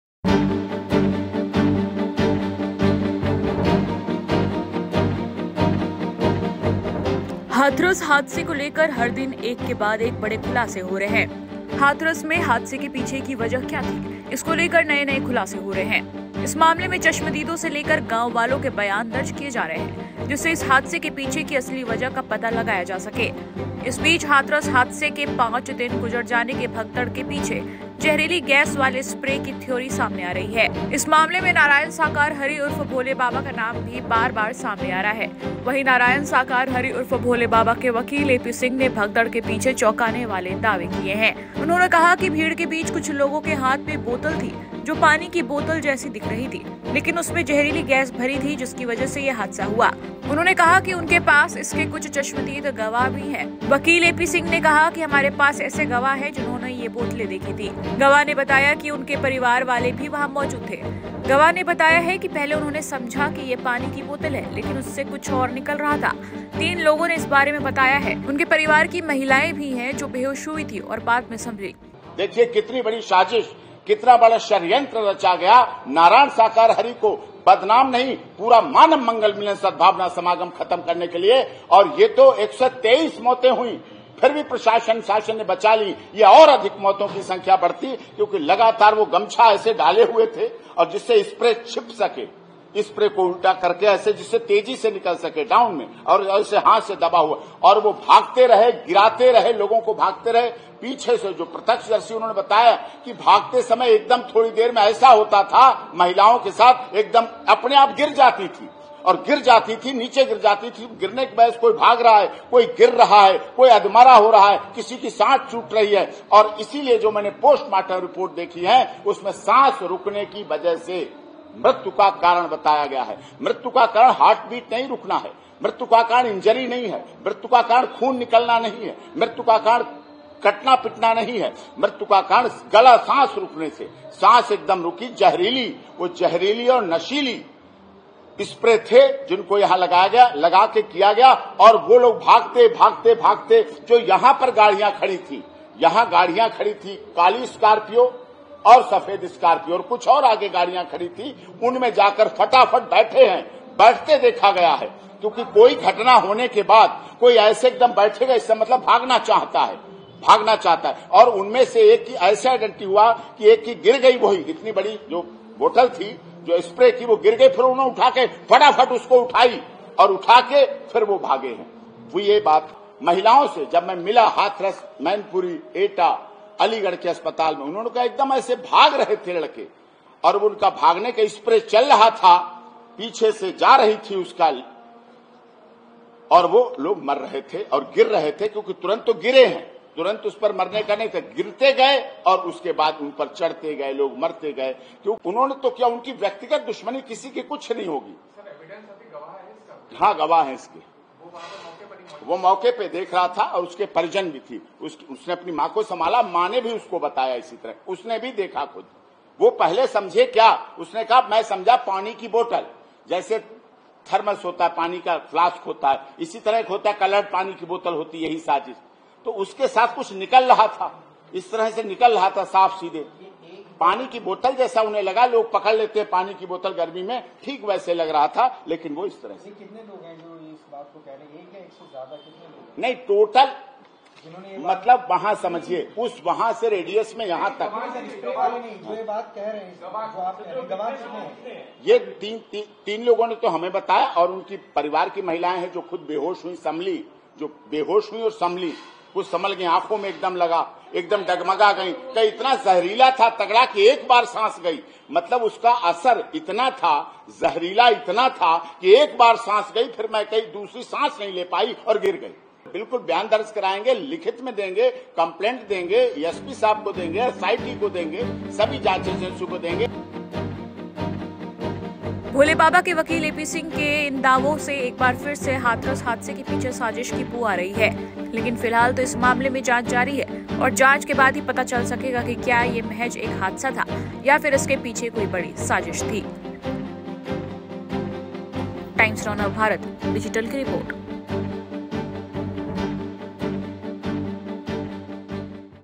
Hathras Stampede News: हाथरस में हादसे के पीछे साजिश का खुलासा!| Hindi News
Hathras Stampede News: हाथरस में 2 जुलाई को एक सत्संग के दौरान भगदड़ मचने से 120 से ज्यादा लोगों की मौत के मामले में साजिश को लेकर एक बड़ा दावा किया जा रहा है.भोले बाबा के वकील एपी सिंह ने कहा है कि इस मामले में लोगों पर जहरीली गैस स्प्रे किया गया जिससे लोगों की हालत बिगड़ी.देखें वीडियो.